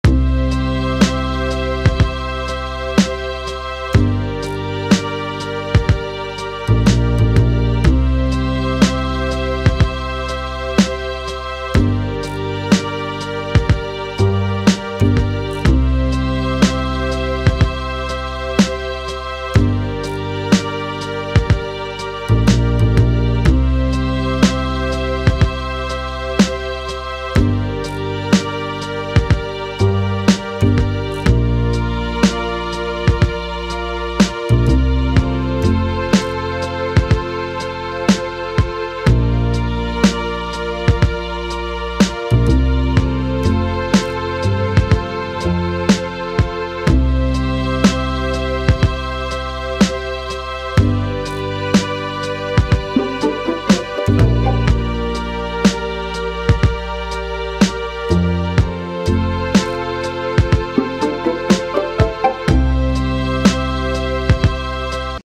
Peter's exercise time
A short video of my Betta Half-moon Mustard male "flaring" at his exercise mirror. 😊